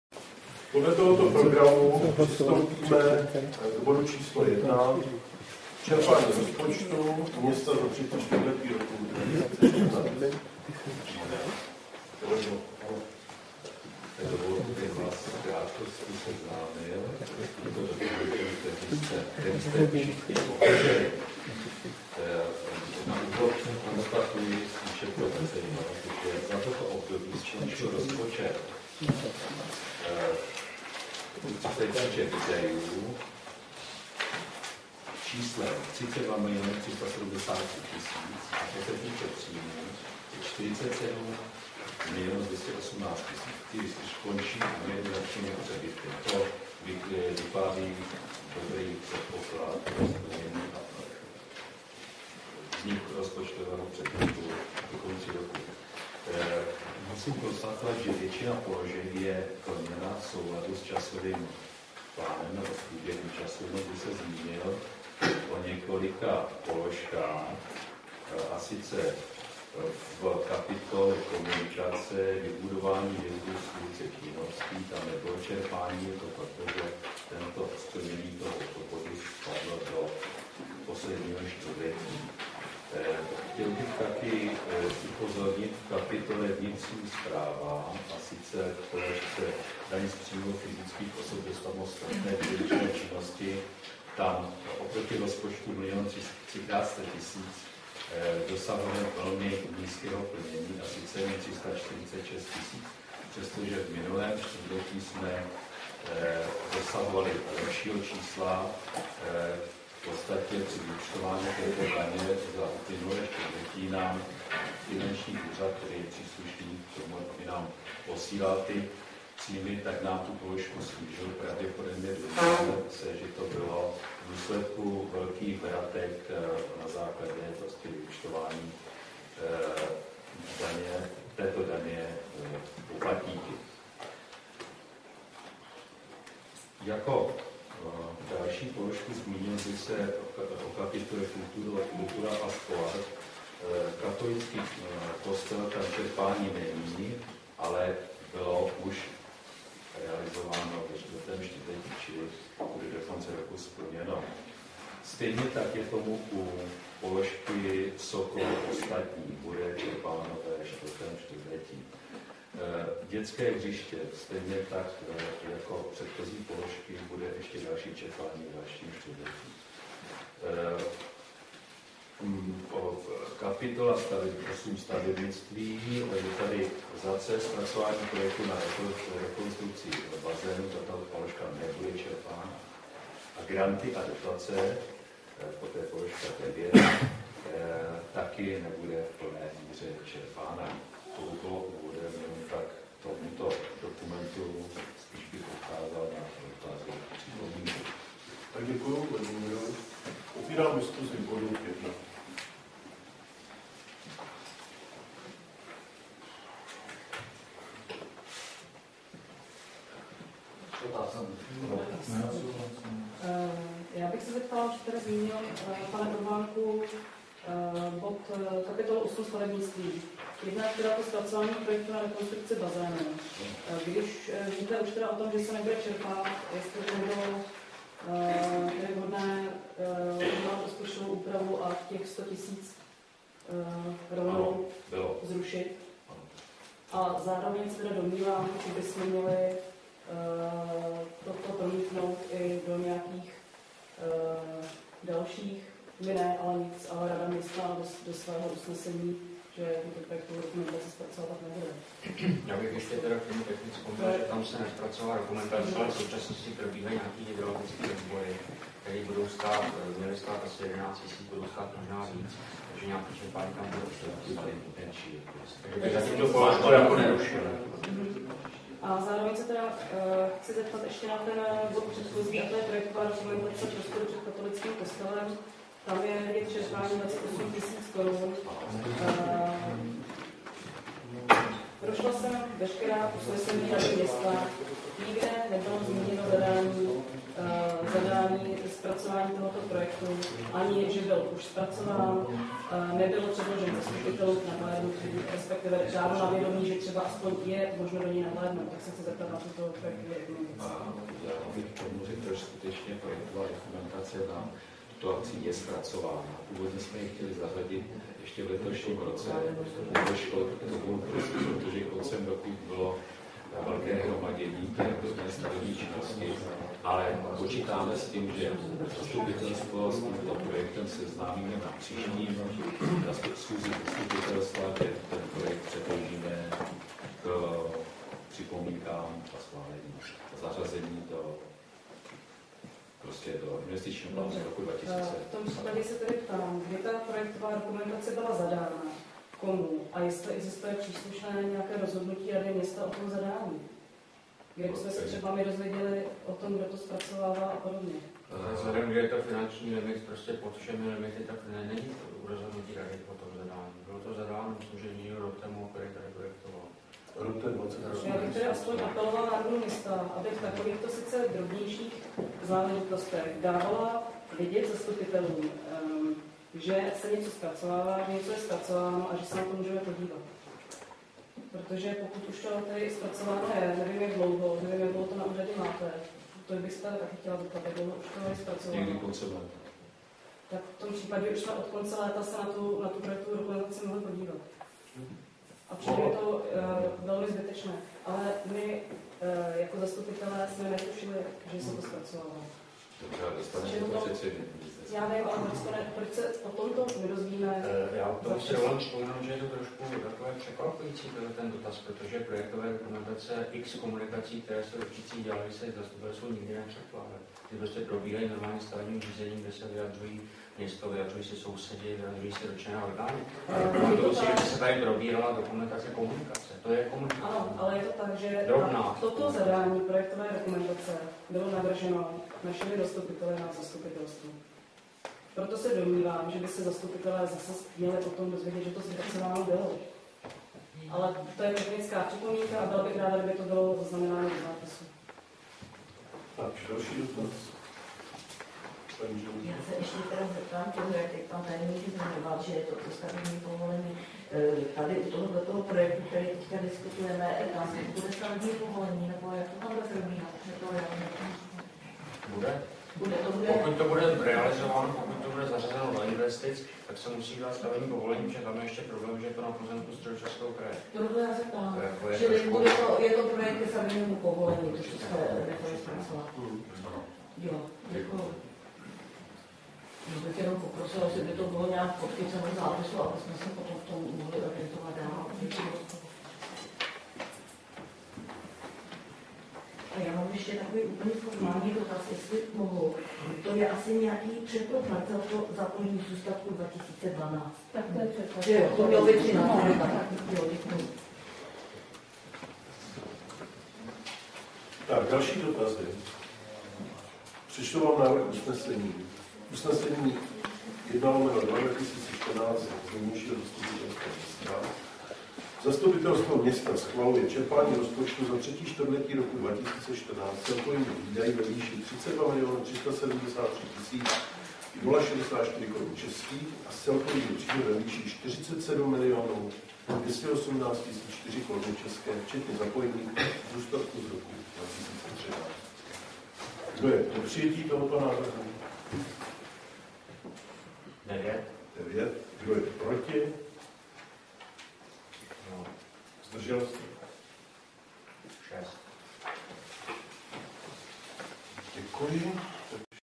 04 Čerpání rozpočtu za III čtvrtletí 2014
Záznam zasedání zastupitelstva Libčice nad Vltavou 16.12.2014
Čerpání rozpočtu za III čtvrtletí 2014